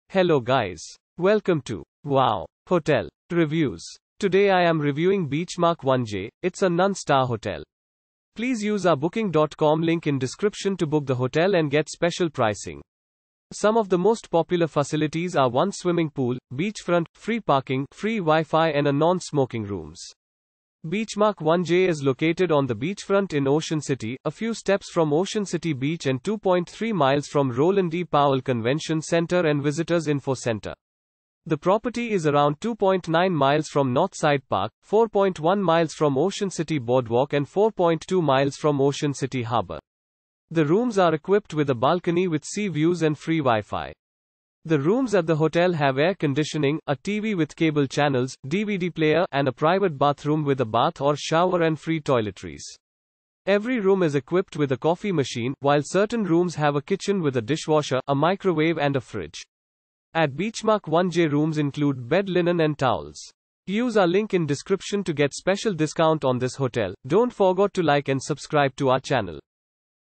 Beachmark 1J Review - Ocean City , United States of America
Beachmark 1J

Beachmark 1J is located on the beachfront in Ocean City, a few steps from Ocean City Beach and 2.3 miles from Roland E. Powell Convention Center & Visitors Info Center. The property is around 2.9 miles from Northside Park, 4.1 miles from Ocean City Boardwalk and 4.2 miles from Ocean City Harbor. The rooms are equipped with a balcony with sea views and free WiFi.
The rooms at the hotel have air conditioning, a TV with cable channels, DVD-Player, and a private bathroom with a bath or shower and free toiletries. Every room is equipped with a coffee machine, while certain rooms have a kitchen with a dishwasher, a microwave and a fridge. At Beachmark 1J rooms include bed linen and towels.